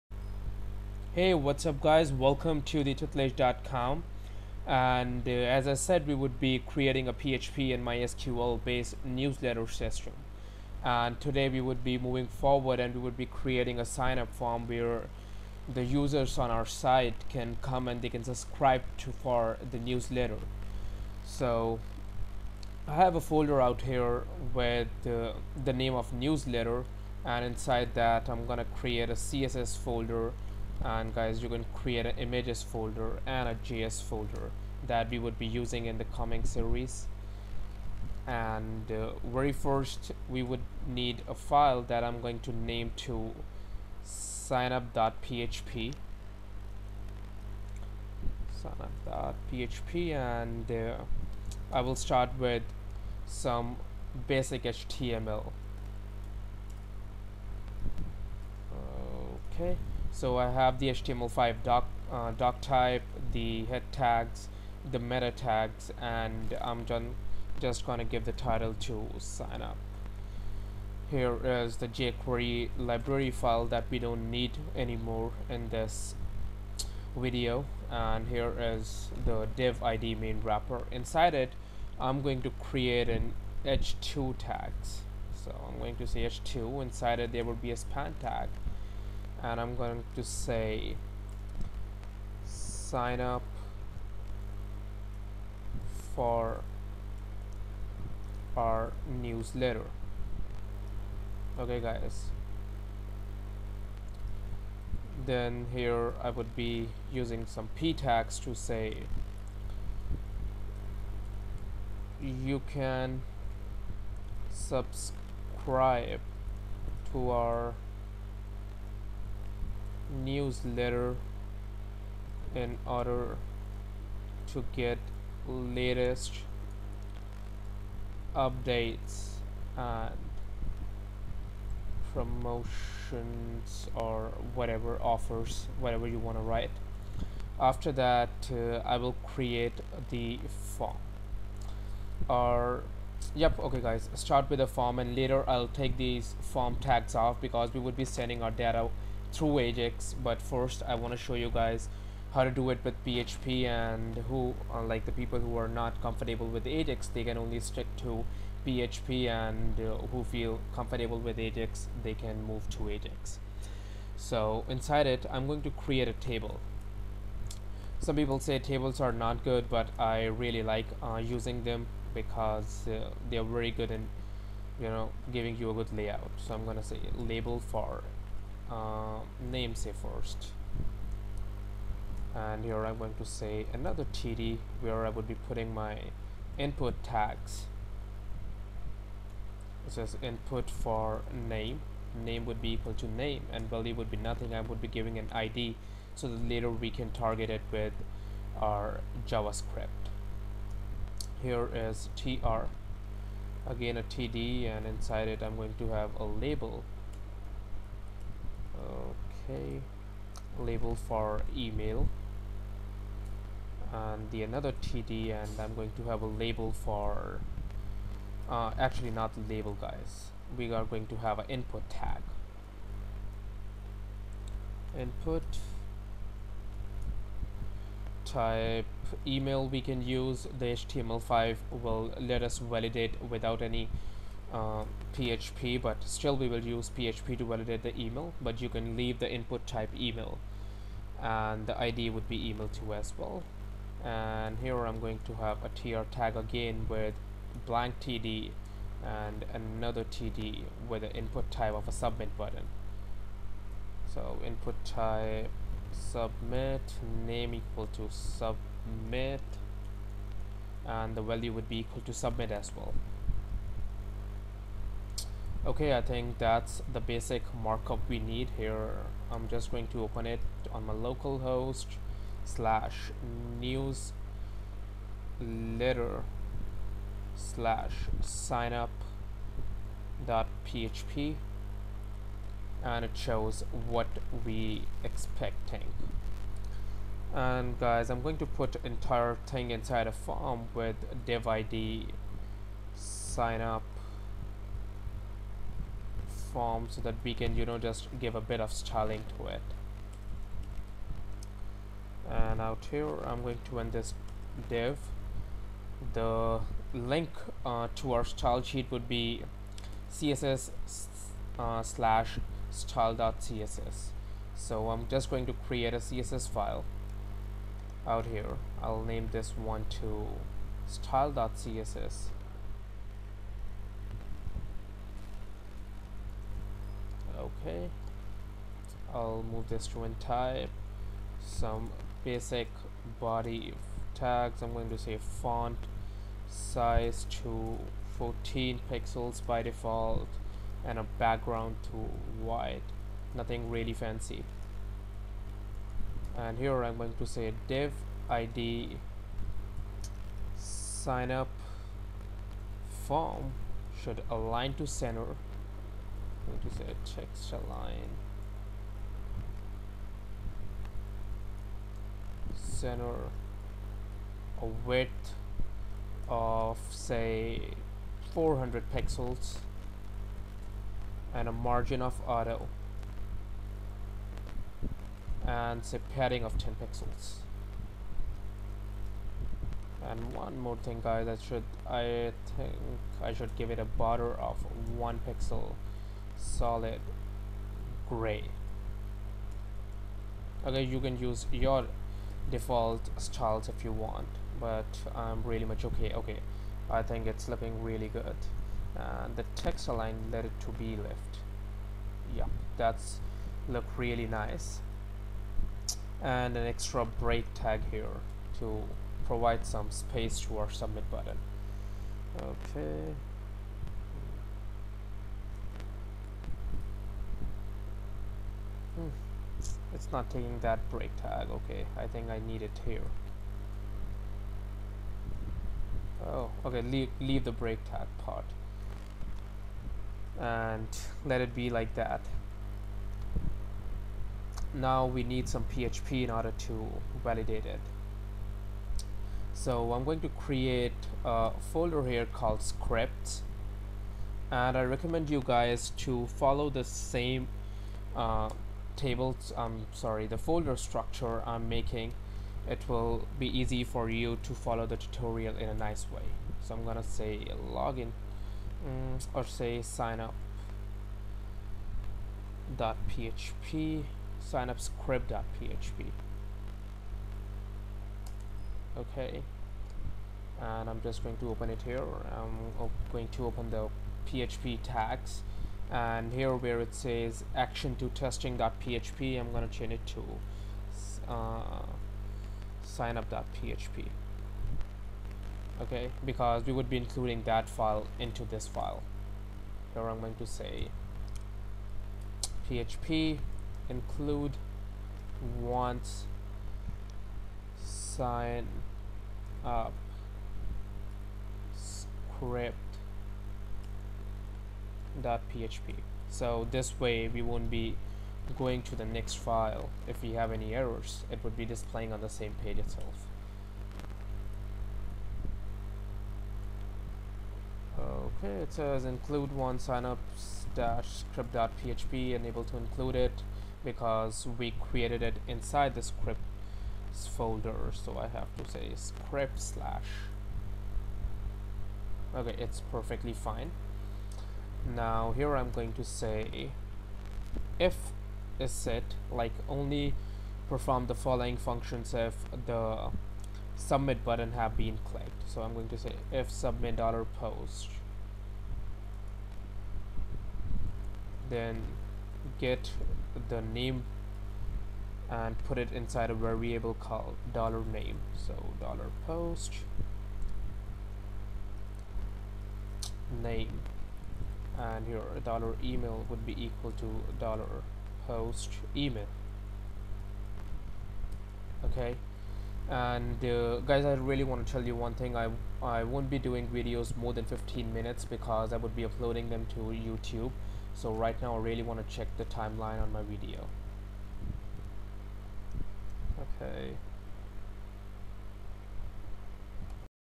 Php and Mysql Based Newsletter System Part 2.1:- Setting up the signup form
In this part i will show to guys how to make a signup form in php and then make a entry to our database with user information